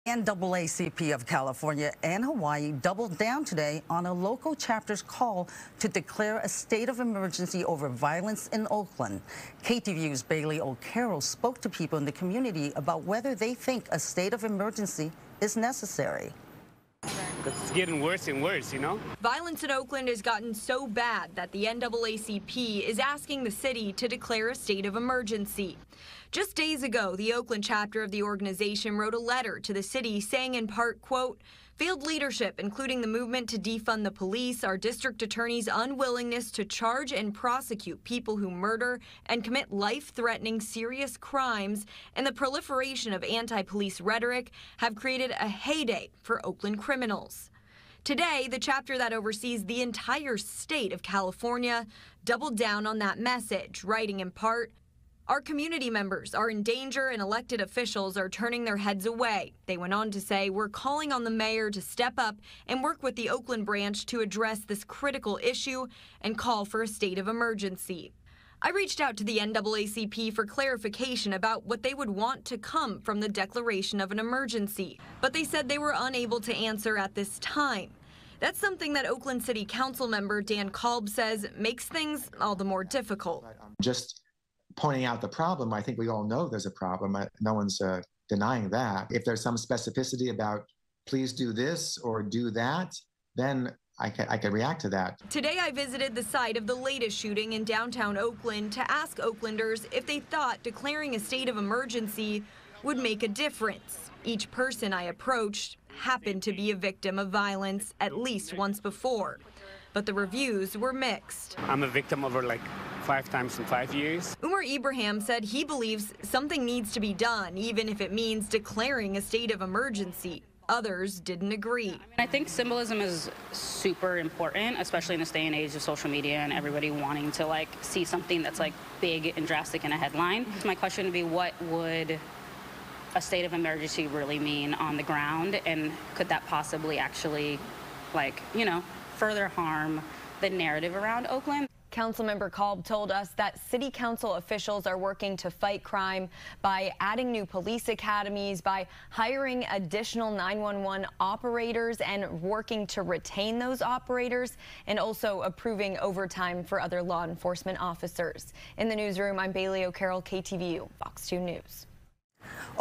Regional NAACP doubles down on Oakland chapter's request for state of emergency declaration over ong
Violence in Oakland has gotten so bad, that NAACP's Oakland chapter asked that the city declare a state of emergency, and now, the regional branch is doubling down.